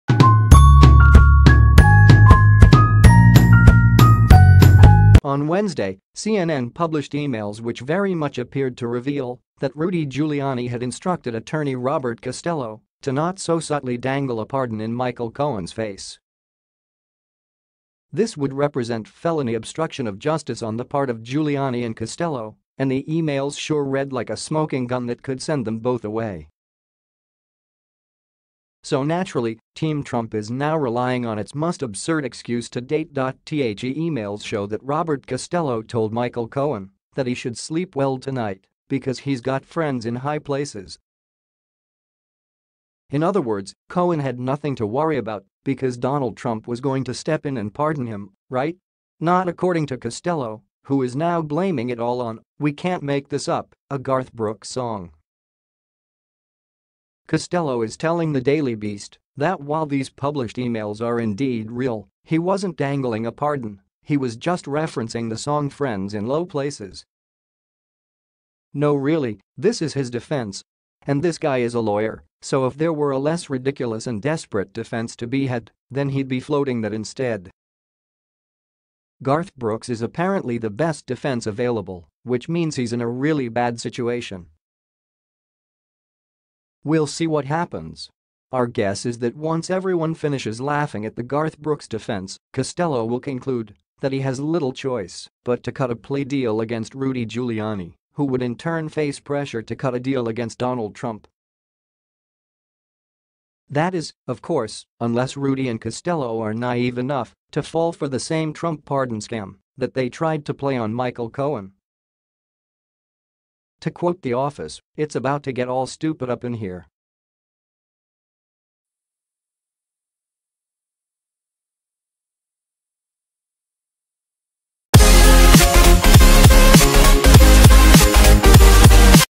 CNN published emails which very much appeared
On Wednesday, CNN published emails which very much appeared to reveal that Rudy Giuliani had instructed attorney Robert Costello to not so subtly dang